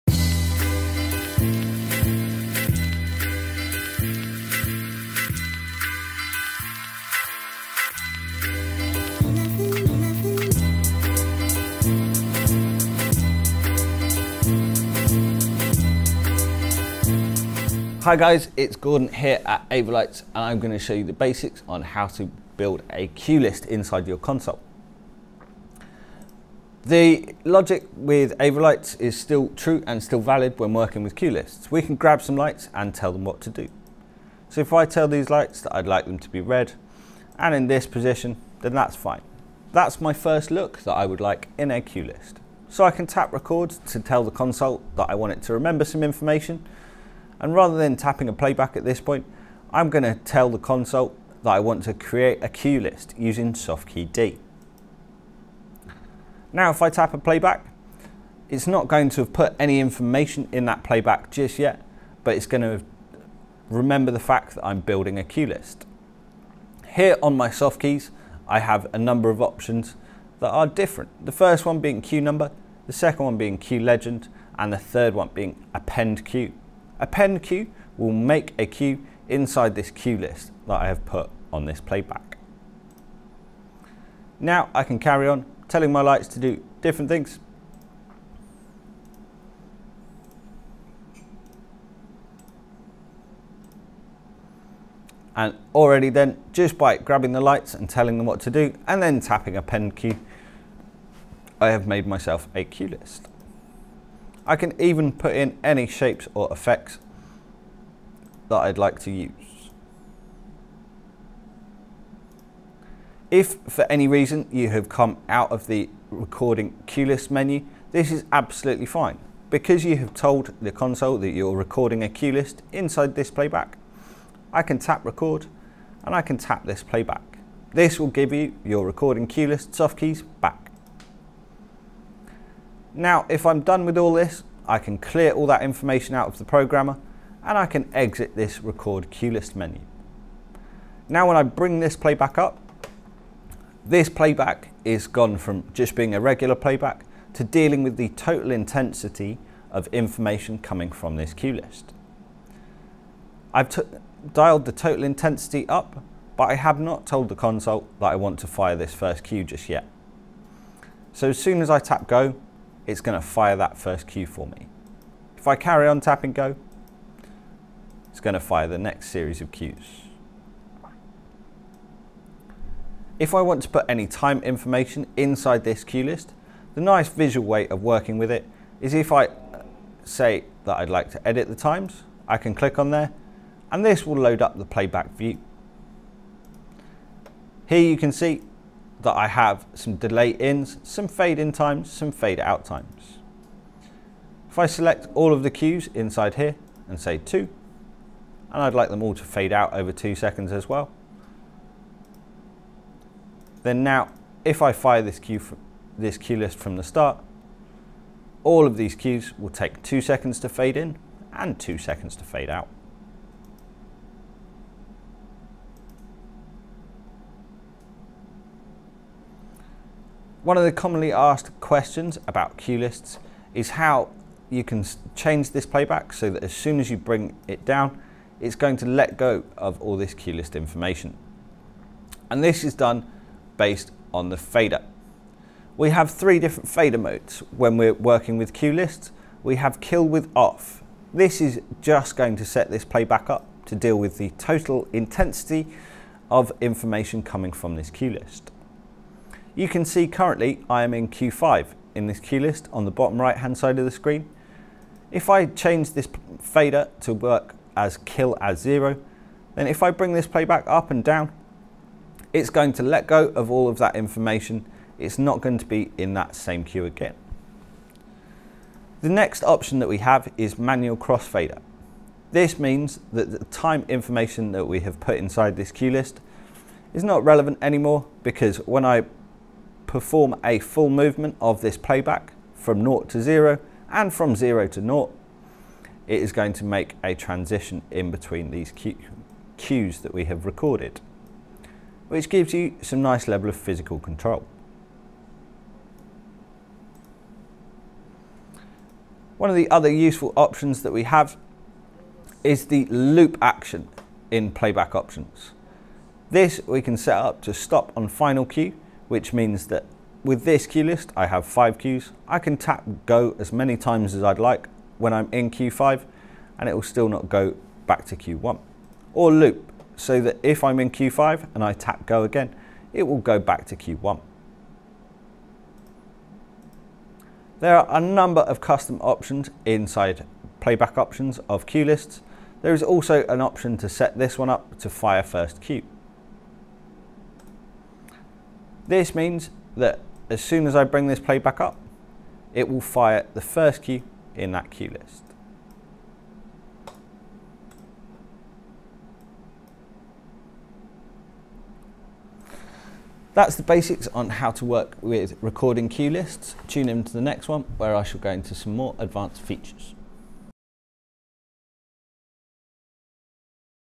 Cue list Basics
Follow this step by step guide to add simple cuelists in your Titan Software.